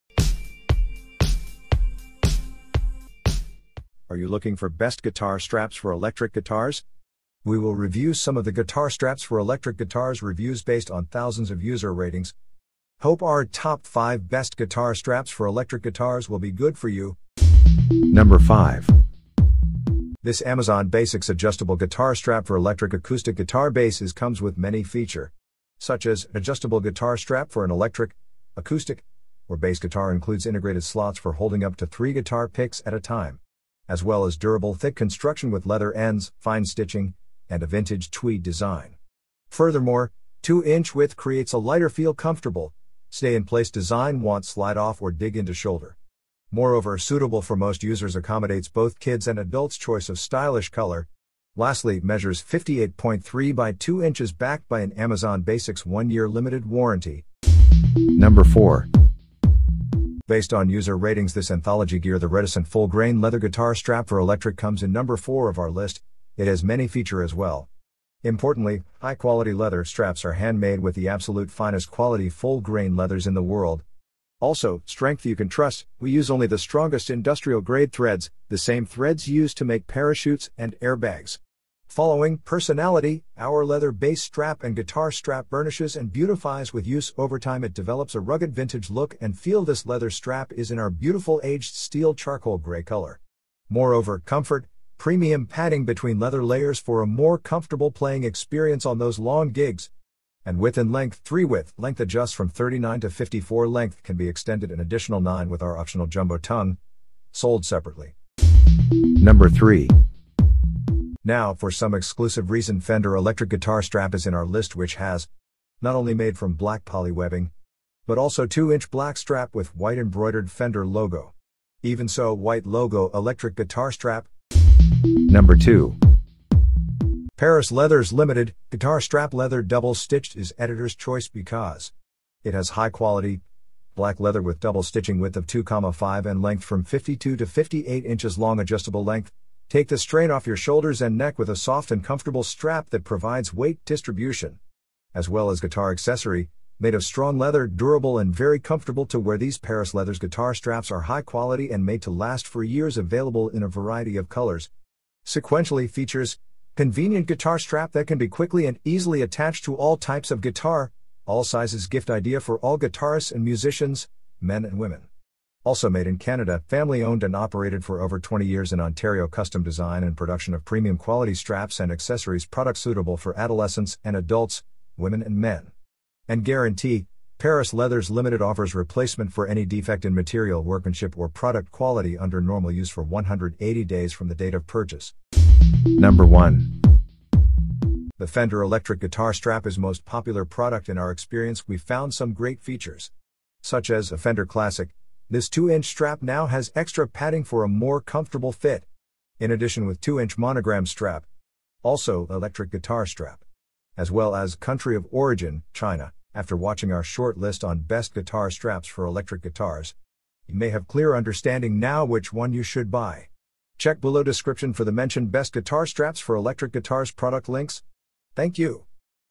Top 5 Best Guitar Straps For Electric Guitars - To Buy Online
Best Guitar Straps For Electric Guitars: In this video we've collected and suggested  Guitar Straps For Electric Guitars Reviews for you. Hope our findings will help you.

✰ Fender Electric Guitar Strap:
✰ Perri’s Leathers Ltd., Guitar Strap – Leather – Double Stitched:
✰ Anthology Gear"The Reticent" Full Grain Leather Guitar Strap For Electric:
✰ Amazon Basics Adjustable Guitar Strap For Electric/Acoustic Guitar/Bass:

►Timestamps◄
 ⏩ 0:00:03 Introduction
 ⏩ 00:00:18 Amazon Basics Adjustable Guitar Strap For Electric/Acoustic Guitar/Bass
 ⏩ 00:00:57 Anthology Gear"The Reticent" Full Grain Leather Guitar Strap For Electric
 ⏩ 00:01:47 Fender Electric Guitar Strap
 ⏩ 00:02:02 Perri’s Leathers Ltd., Guitar Strap – Leather – Double Stitched
 ⏩ 00:03:01 Fender Electric Guitar Strap
 ⏩ 00:03:21 Conclusion.